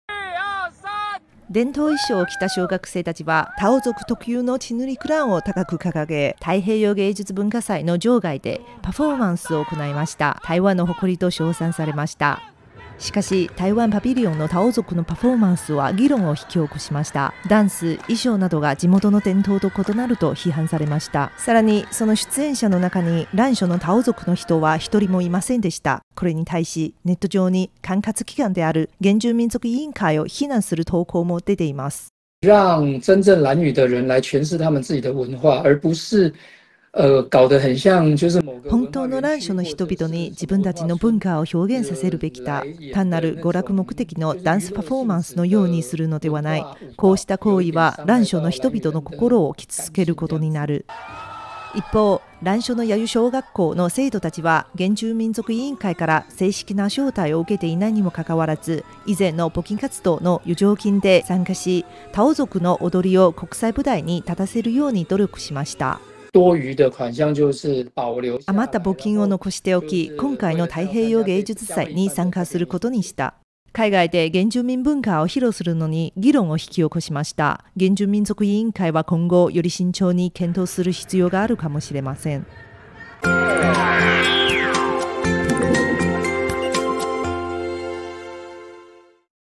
太平洋芸術文化祭で現地住民不在の原住民公演、蘭嶼の長老が不満
「第13回太平洋芸術文化祭」は6月6日から16日までアメリカのハワイで盛大に開催され、台湾からも多くの原住民団体が招待されて出演した。しかし、台湾東部・台東県の離島、蘭嶼の人々は、今回「台湾パビリオン」の舞台で出演するタオ族の団体に地元の人々が全くおらず、さらにそのパフォーマンスの歌や踊りが地元の伝統と異なったことに気付いた。

一方、蘭嶼の椰油国民小学校の生徒たちは、先生の指導のもと、募金で集めたお金で自費で現地に渡り、パフォーマンスを行った。この子供たちの会場外でのパフォーマンスは「台湾の光（台湾の誇り）」だと称賛された。しかし、団体を率いる先生は、なぜ、台湾の原住民族の行政を担う「原住民族委員会」が、地元の住民を「台湾パビリオン」のパフォーマンスに起用しなかったのかと嘆いているという。